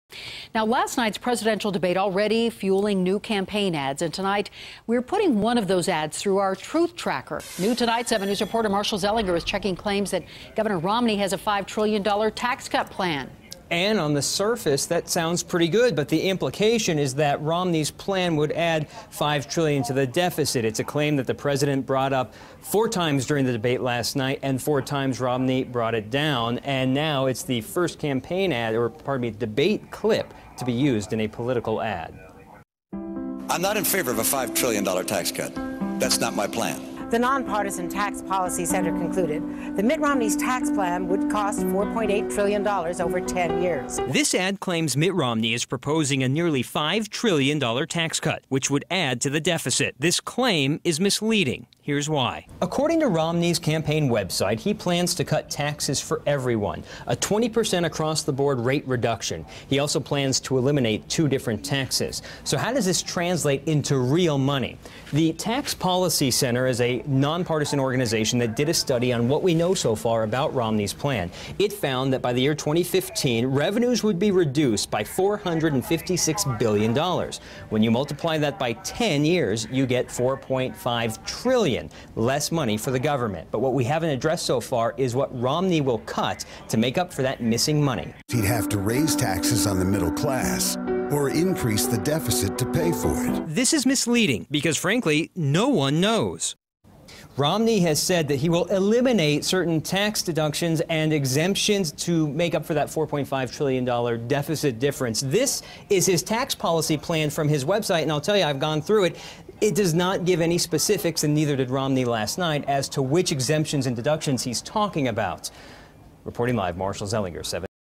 Truth Tracker: Does Romney have a $5 trillion tax cut plan?
7NEWS is putting campaign ads to the test.  Marshall Zelinger reports.